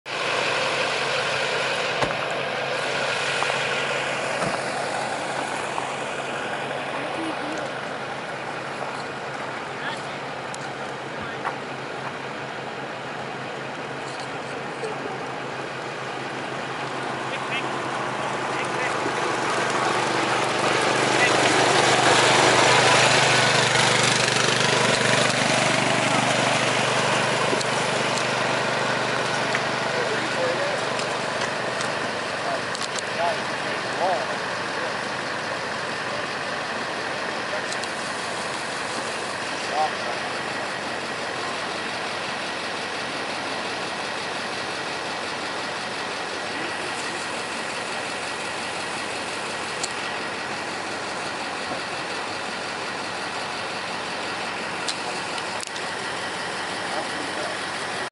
Sled Dogs In Training
Sled dogs in training for the upcoming dog sled races in 2016/2017 winter season. Video taken in Paxson, Alaska.